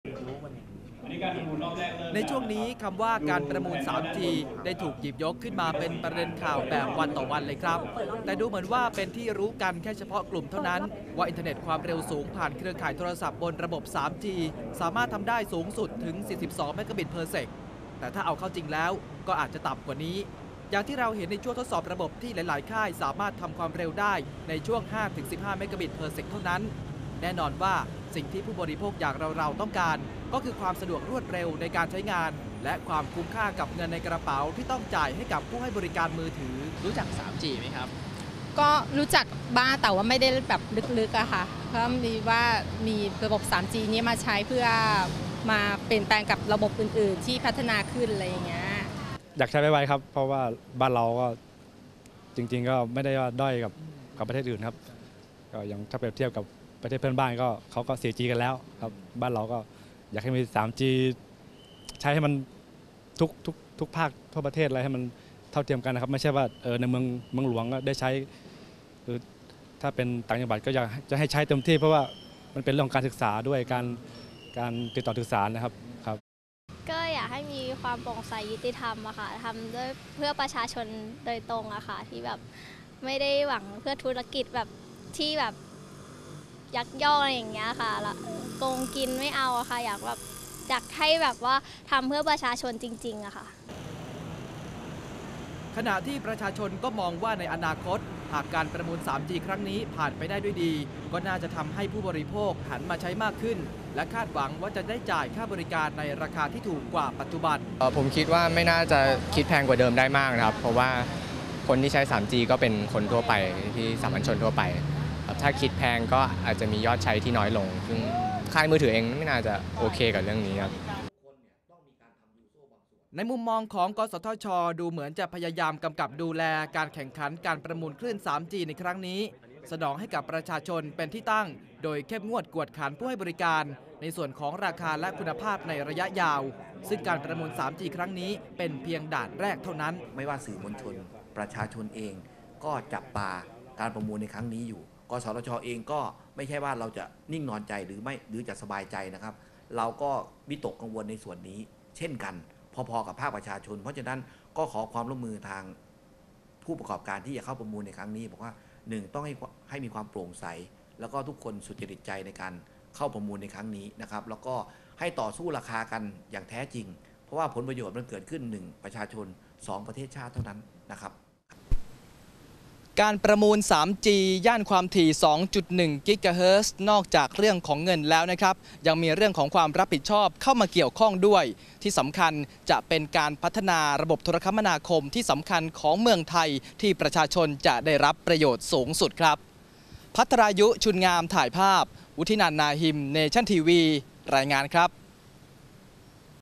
คนไทยเข้าใจ 3G แล้วหรือยัง?
การประมูล 3G ก็เข้าใกล้ขึ้นเรื่อยๆ แล้ว แต่ก่อนจะสรุปความเข้าใจแบบรวบยอดถึงการประมูล 3G ที่กำลังจะมีขึ้นในบ้านเรา ไปฟังความคิดเห็นของภาคประชาชนกันหน่อย ว่าเค้าเข้าใจระบบ3จี และคาดหวังอะไรกับ 3G ย่าน 2.1 GHz  ไปตรวจสอบกับคุณวุฒินันท์ นาฮิม ครับ

นักข่าว วุฒินันท์ นาฮิม
ช่างภาพ  ณพกร  ยงทอง
ตัดต่อ กฤษดา เอกอัครบัณฑิต พิเชฐ พิชิตมารกุล